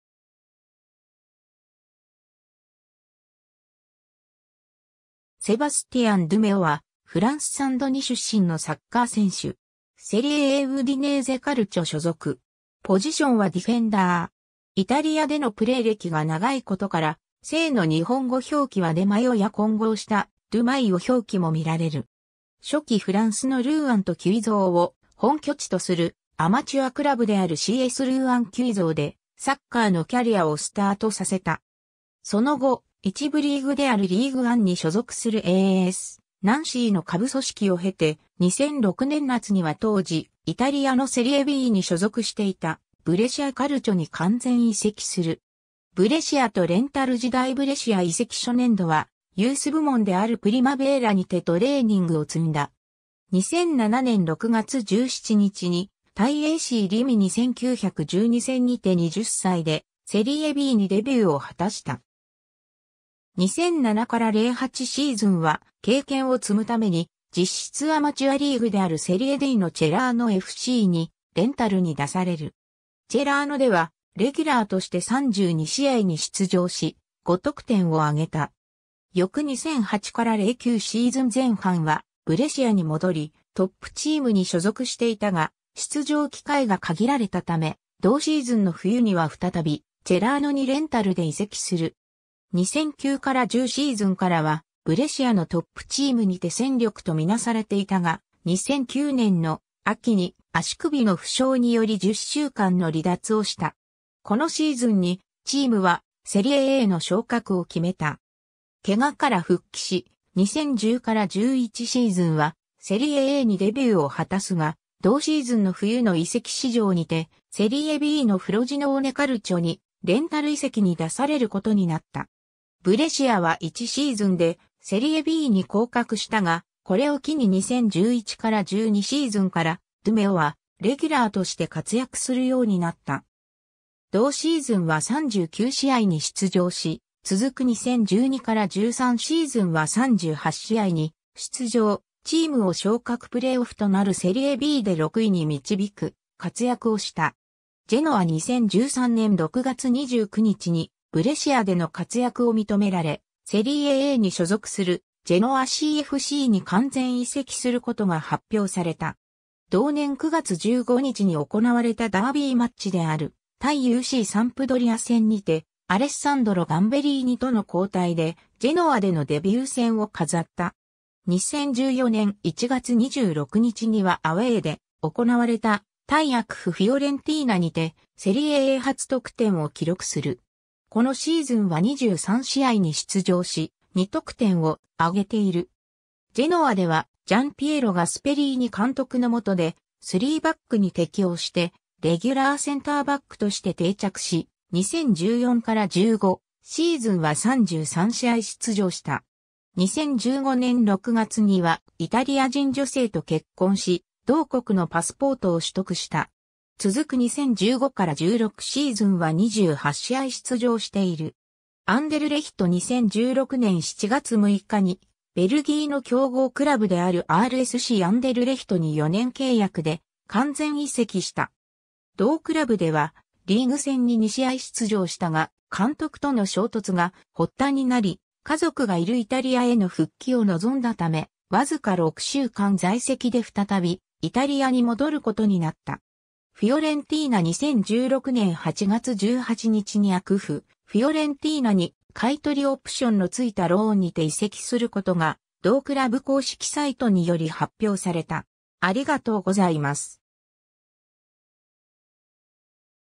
セバスティアン・ドゥ・メオ
ブレシア・カルチョの選手
フロジノーネ・カルチョの選手
セバスティアン・ドゥ・メオ（フランス語: Sebastian De Maio, 1987年3月5日 - ）は、フランス・サン＝ドニ出身のサッカー選手。
セリエA・ウディネーゼ・カルチョ所属。
イタリアでのプレー歴が長いことから、姓の日本語表記はデ・マイオや混合したドゥ・マイオ表記もみられる。
 初期 フランスのルーアンとキュイゾーを本拠地とするアマチュアクラブであるCSルーアン=キュイゾーでサッカーのキャリアをスタートさせた。
その後、1部リーグであるリーグ・アンに所属するASナンシーの下部組織を経て、2006年夏には当時イタリアのセリエBに所属していたブレシア・カルチョに完全移籍する。
 ブレシアとレンタル時代 ブレシア移籍初年度はユース部門であるプリマヴェーラにてトレーニングを積んだ。
2007年6月17日に対ACリミニ1912戦にて20歳でセリエBにデビューを果たした。
2007-08シーズンは経験を積むために実質アマチュアリーグであるセリエDのチェラーノFCにレンタルに出される。
チェラーノではレギュラーとして32試合に出場し、5得点をあげた。
翌2008-09シーズン前半はブレシアに戻りトップチームに所属していたが出場機会が限られたため、同シーズンの冬には再びチェラーノにレンタルで移籍する。
2009-10シーズンからはブレシアのトップチームにて戦力とみなされていたが、2009年の秋に足首の負傷により10週間の離脱をした。
このシーズンにチームはセリエAの昇格を決めた。
怪我から復帰し、2010-11シーズンはセリエAにデビューを果たすが、同シーズンの冬の移籍市場にてセリエBのフロジノーネ・カルチョにレンタル移籍に出されることになった。
ブレシアは1シーズンでセリエBに降格したが、これを機に2011-12シーズンからドゥ・メオはレギュラーとして活躍するようになった。
同シーズンは39試合に出場し、続く2012-13シーズンは38試合に出場、チームを昇格プレーオフとなるセリエBで6位に導く活躍をした。
 ジェノア 2013年6月29日に、ブレシアでの活躍を認められセリエAに所属するジェノアCFCに完全移籍することが発表された。
同年9月15日に行われたダービーマッチである対UCサンプドリア戦にて、アレッサンドロ・ガンベリーニとの交代でジェノアでのデビュー戦を飾った。
2014年1月26日にはアウェイで行われた対ACFフィオレンティーナにてセリエA初得点を記録する。
このシーズンは23試合に出場し、2得点をあげている。
ジェノアではジャン・ピエロ・ガスペリーニ監督の下で3バックに適応してレギュラーセンターバックとして定着し、2014-15シーズンは33試合出場した。
2015年6月にはイタリア人女性と結婚し、同国のパスポートを取得した。
続く2015-16シーズンは28試合出場している。
 アンデルレヒト 2016年7月6日にベルギーの強豪クラブであるRSCアンデルレヒトに4年契約で完全移籍した。
同クラブではリーグ戦に2試合出場したが、監督との衝突が発端になり家族がいるイタリアへの復帰を望んだため、わずか6週間在籍で再びイタリアに戻ることになった。
 フィオレンティーナ 2016年8月18日にACFフィオレンティーナに買取オプションの付いたローンにて移籍することが同クラブ公式サイトにより発表された。